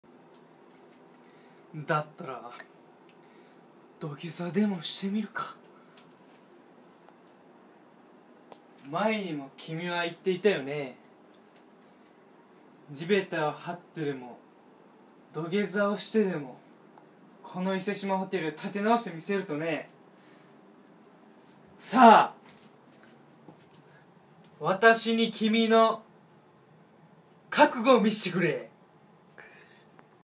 半沢直樹 名シーン 覚悟を見せてくれ
半沢直樹の名シーンをた○おかこー○ーくんが再現しました。カメラマンはktmr